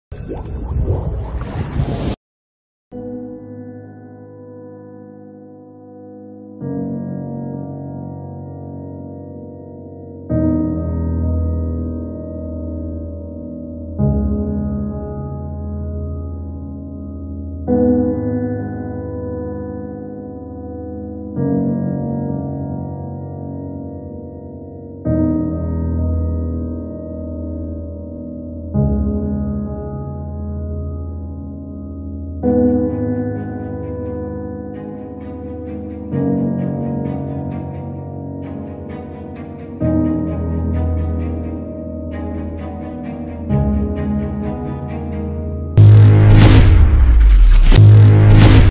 Cerita Andi Lindungi ayah Dari Atap Ambruk di RSAL Dr Ramelan Surabaya
Andi sedang menunggu ayahnya menjalani perawatan uap. namun tiba-tiba terdengar suara gemuruh dari atas, secara spontan, Andi melindungi sang ayah dari reruntuhan atap

Rsedang menunggu ayahnya menjalani perawatan uap. namun tiba-tiba terdengar suara gemuruh dari atas, secara spontan, Andi melindungi sang ayah dari reruntuhan atap

Video Journalist : Fahriza Tito
Editor : Oktavian Dio